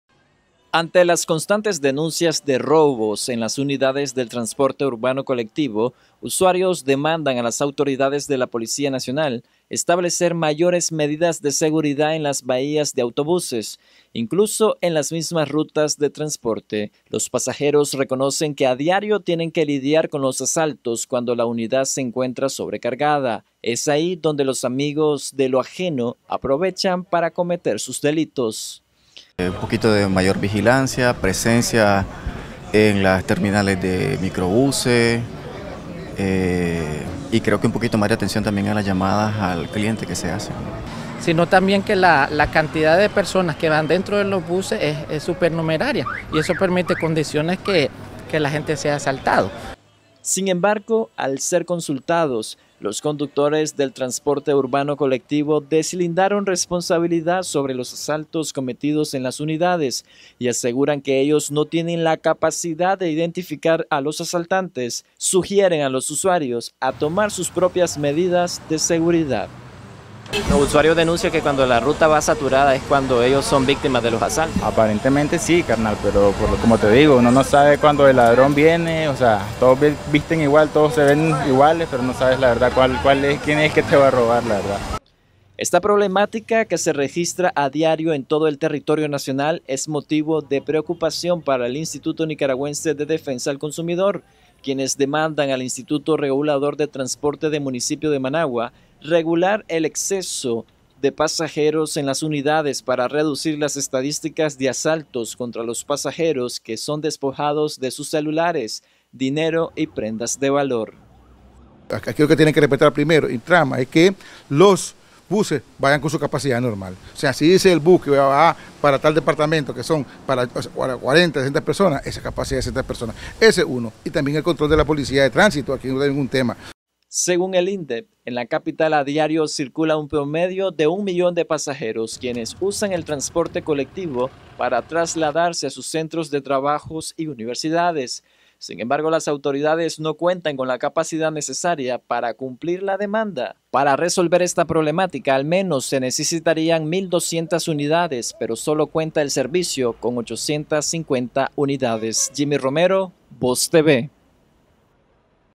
Sobrecarga de pasajeros facilita robos en buses de Managua
La sobrecarga de pasajeros que se registra a diario en las unidades de transporte colectivo de Managua, principalmente en las horas pico, incrementa el número de robos de los cuales son víctimas los capitalinos, según especialistas.